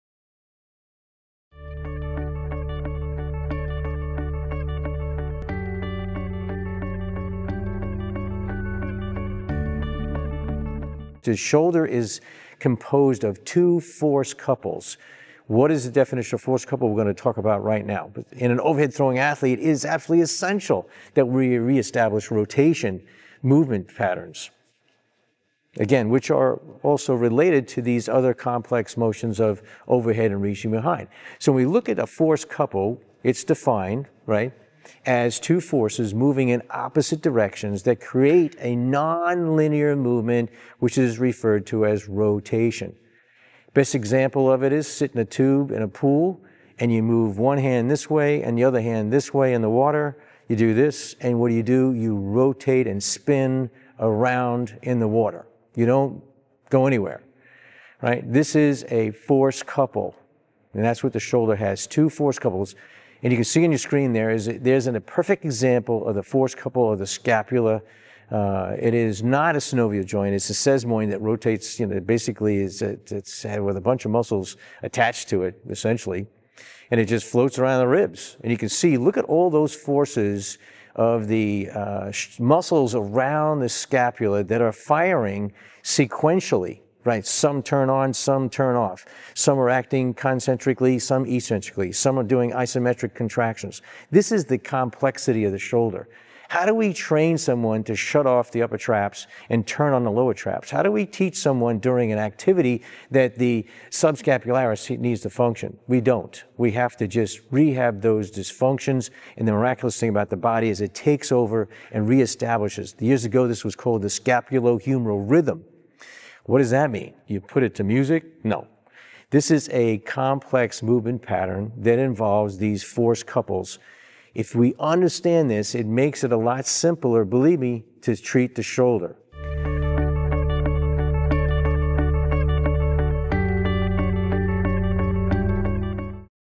The Shoulder's Force Couples - Robert Donatelli - Physical Therapy | MedBridge
Taking care of the shoulder in overhead throwing activities is a complex task. This course provides a detailed review of the mechanics of overhead throwing. Dr. Robert Donatelli further discusses the assessment of the shoulder to determine muscle deficits and soft tissue restrictions such as gross internal rotation deficit. At the end of the course, the student will be able to identify the shoulder mechanics specific to the overhead throwing athlete, and assess the joint posture, muscle alignment, and soft tissue limitations.